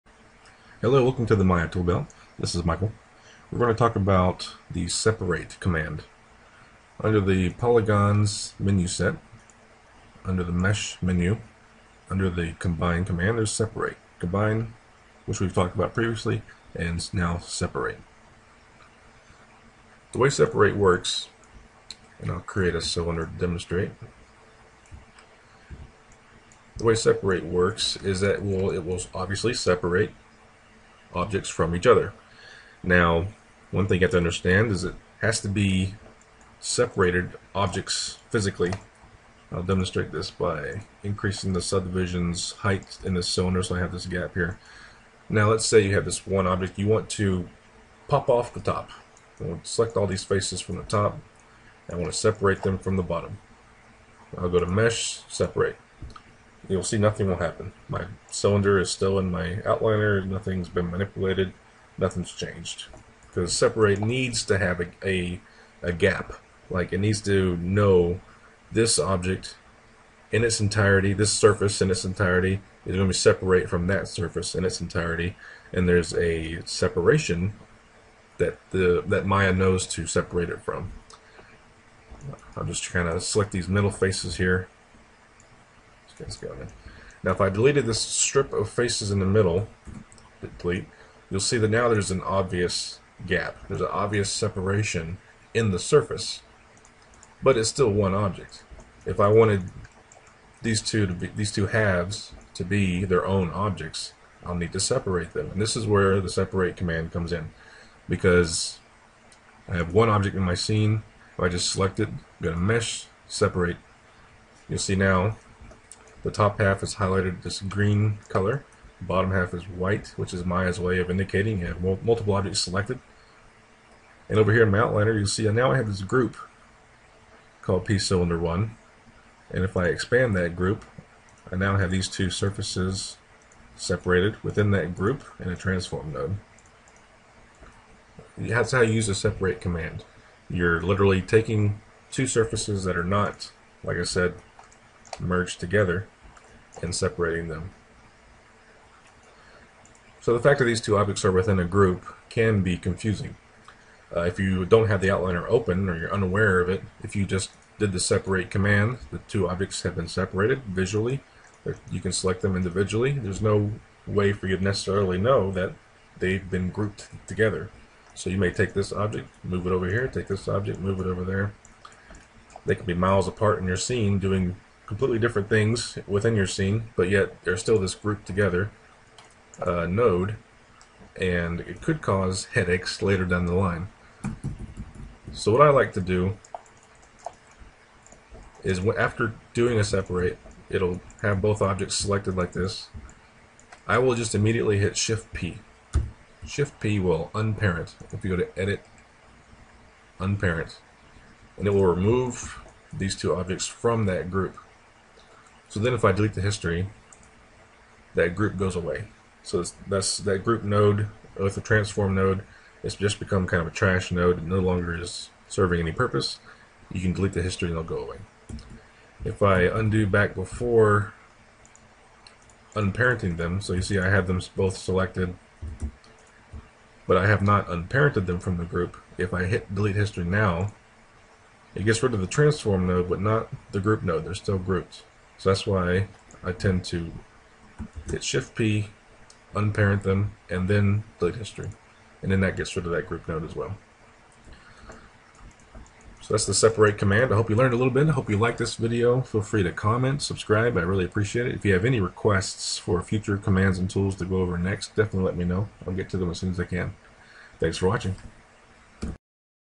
The Maya Toolbelt - Separate
An explanation of Mesh - Separate.

Product Links:
The Game Artist's Guide to Maya
The Game Animator's Guide to Maya
Studio Projects: Environments & Props